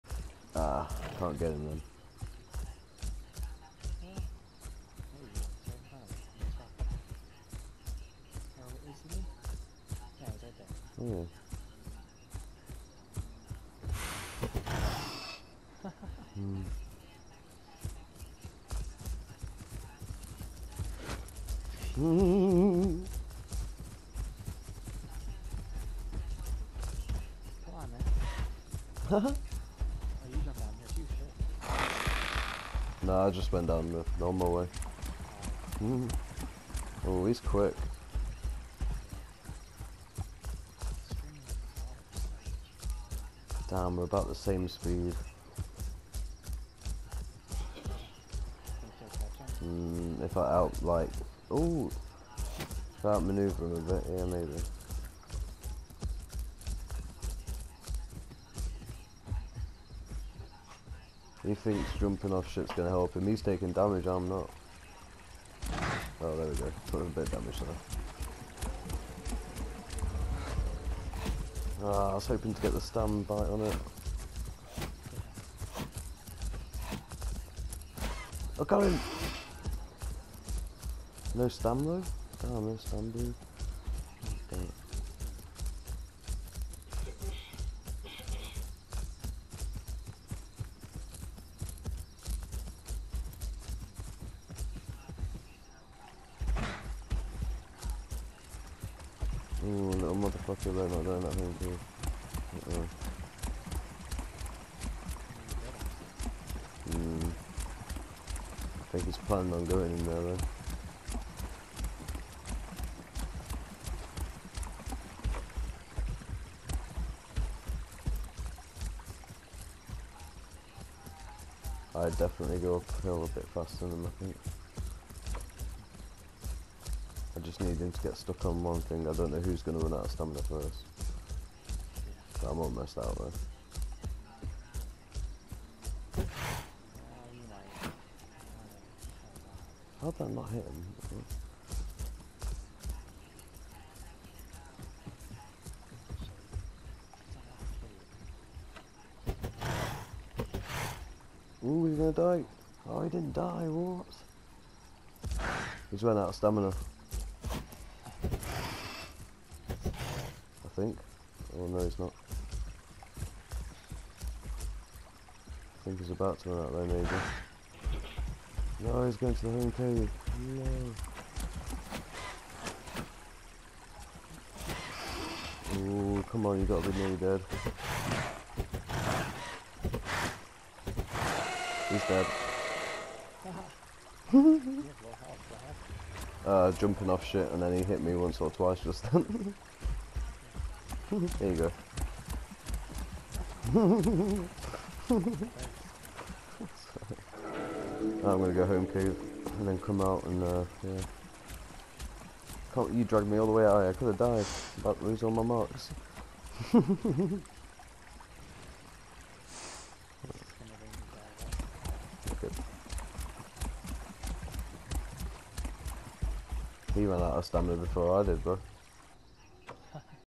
PoT - Run Chicken, Ruuuun!
Adult chicken runs for his life